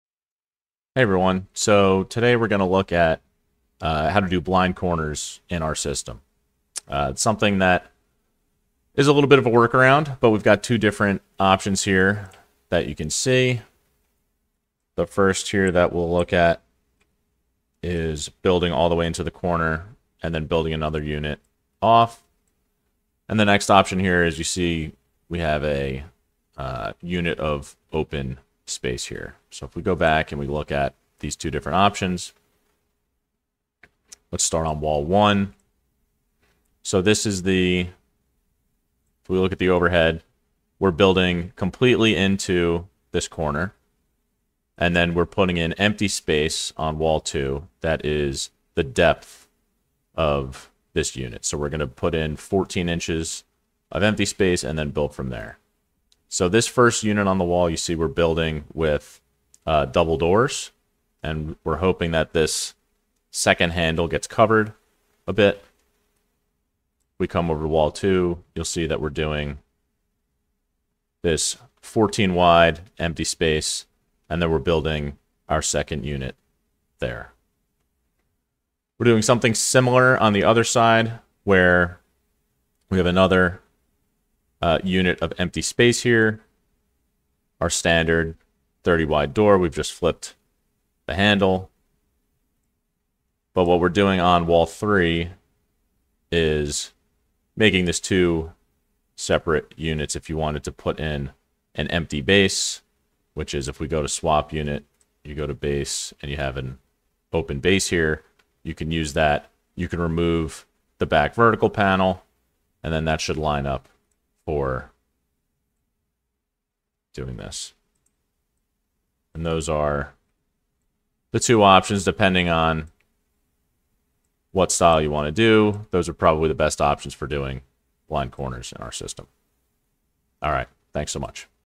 Blind Corners
This video will walk through the different options you have for building blind corners in ClosetPro Software.